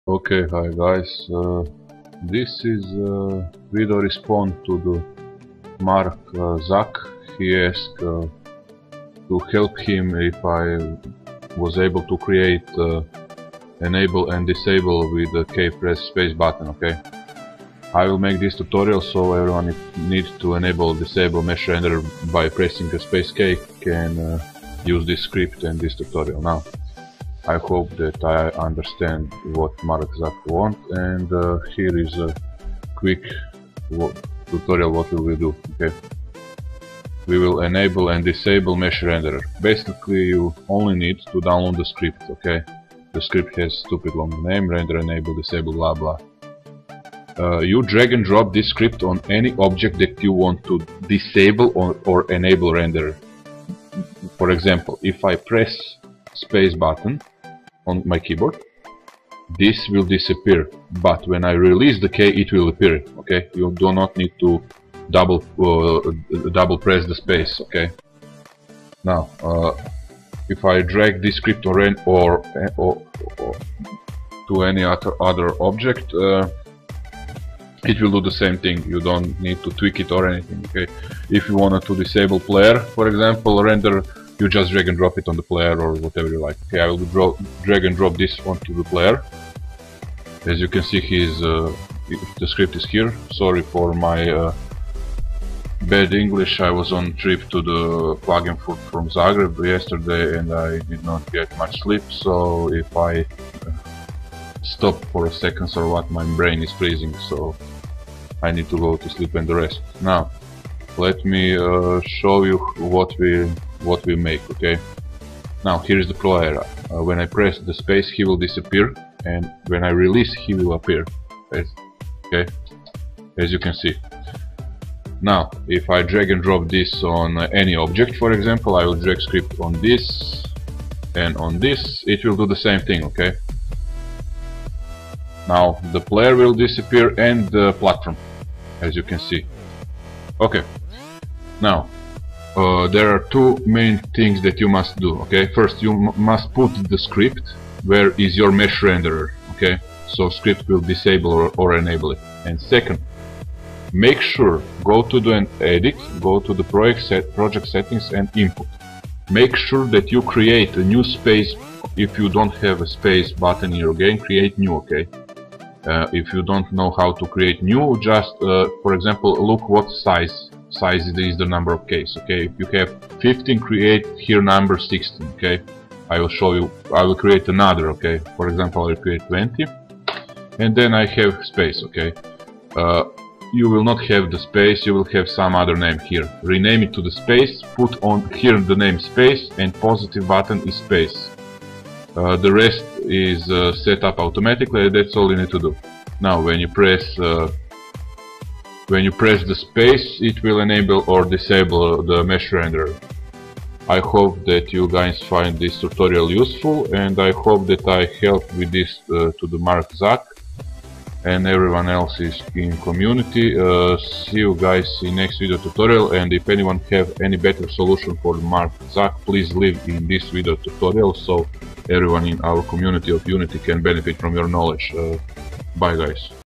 Unity3D Enable and Disable Mesh Rendered With Key
Unity3D Tutorial Enable and Disable Mesh Rendered With  Key

Tutorial 20/24

This is Video Response 2 User : Mark Zuck
Attempt One lol
After 24 hours of no sleep or 4 hours sleep in bus  Max lol
And I try 2 make tutorila 2  Help User after Mark Zuck direct back from my trip Zagreb - klagenfurt - Minimundus - Zagreb
Mega LOL

And Look part 2 lol . Tutorial 21/20 LOL MEGA FAIL  :D

THIS IS Java script: GOOD 4 2D GAMES AND 3D GAMES !

Tutorial 20/24